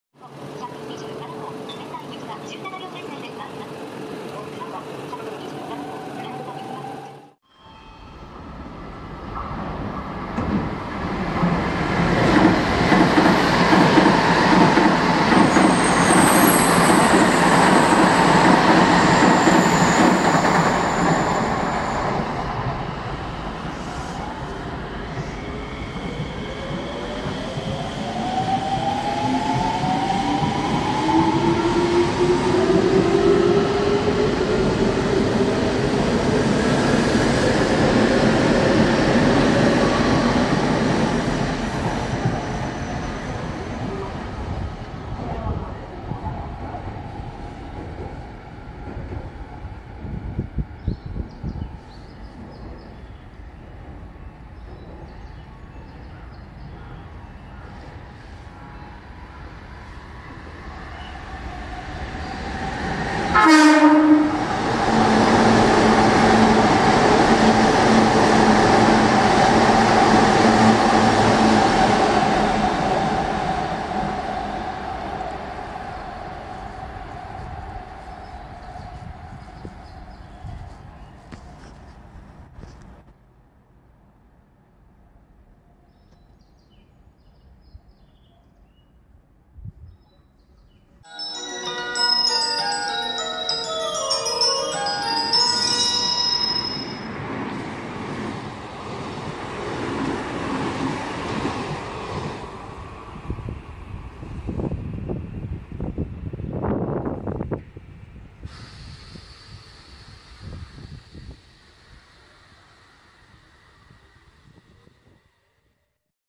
【小田急】複々線区間を走る各駅停車と急行の様子
2059×8
E233-2000　マト3